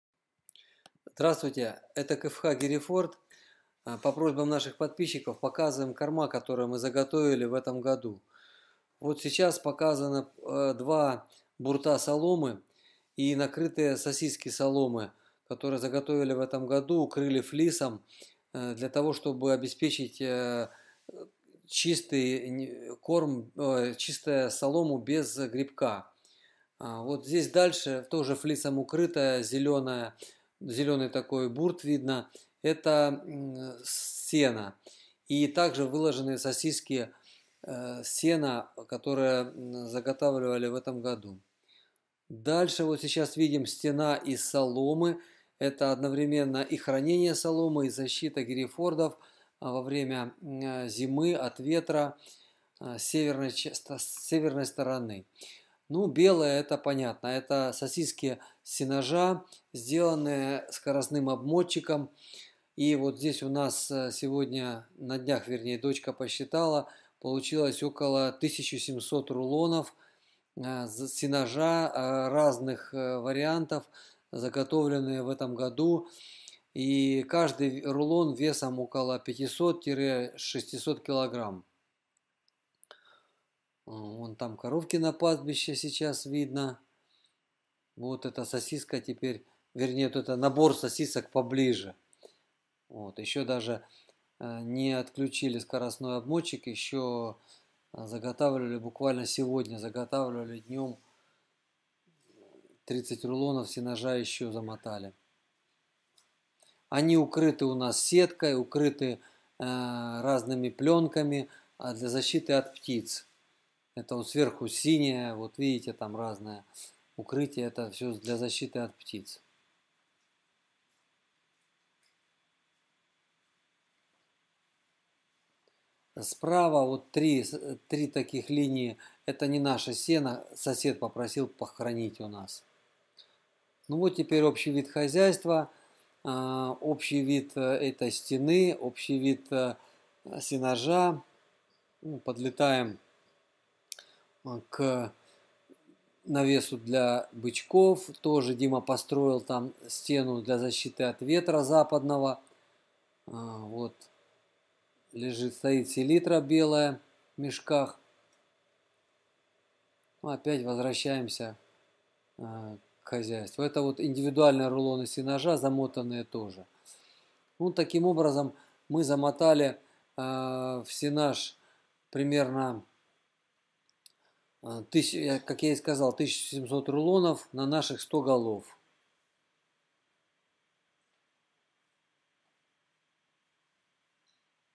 Корма в хозяйстве - осень 2020
Заготовлено около 1700 рулонов сенажа (500-600 кг каждый) - вико-овес-рожь, рожь, клевер со злаковыми, люцерна-ежа, примерно 600 рулонов соломы ржи, пшеницы, овса (около 200 кг каждый), около 300 рулонов сена (250 кг каждый). Общее поголовье сейчас - немного больше 100 голов.